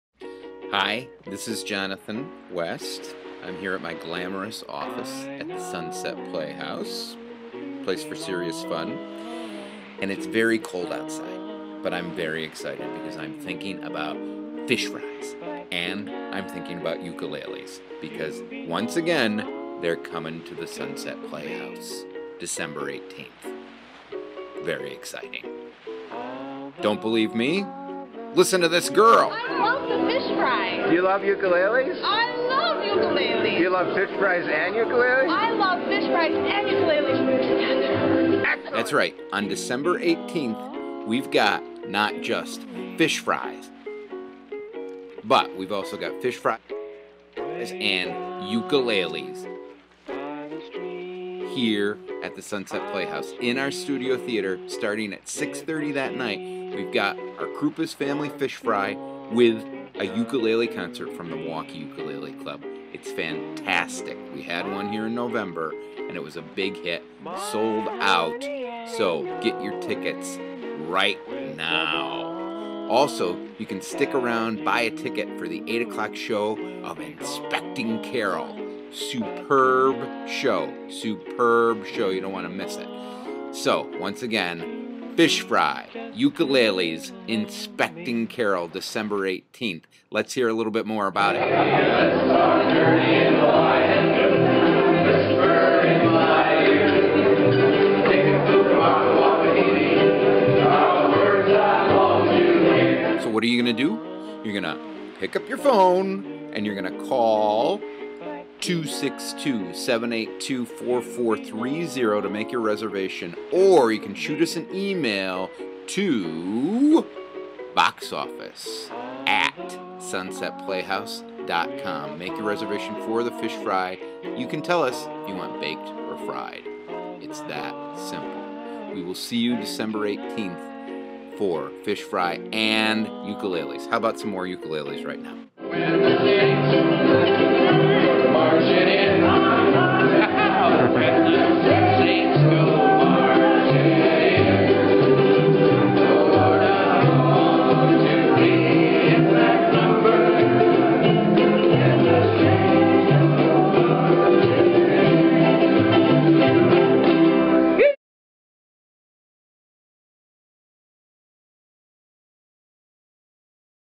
Yum and Strum! Fish Fry and Ukes at Sunset Playhouse
We're having a Fish Fry with a Uke Concert prior to our December 18 performance of INSPECTING CAROL.  10 bucks get you the fish fry and uke tunes.  Buy a ticket for INSPECTING CAROL and hang out for the show after.